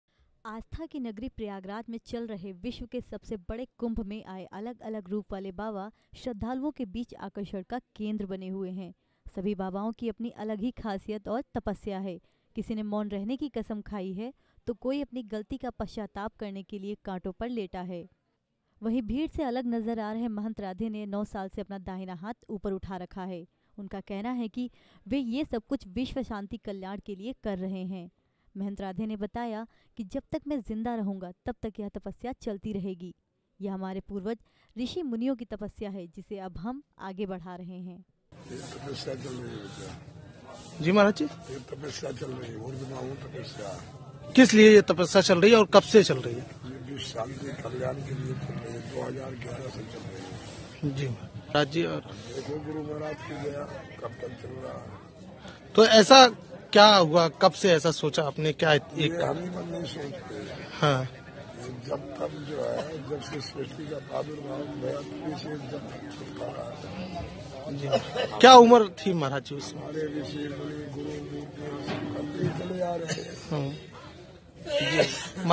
कुंभ स्पेशल : विश्व शांति के लिए इस बाबा ने 9 साल से ऊपर उठा रखा है दाहिना हाथ
प्रयागराज : आस्था की नगरी प्रयागराज में चल रहे विश्व के सबसे बड़े कुंभ में आए अलग-अलग रूप वाले बाबा श्रद्धालुओं के बीच आकर्षण का केंद्र बने हुए हैं. सभी बाबाओं के अपनी एक ही खासियत और तपस्या है.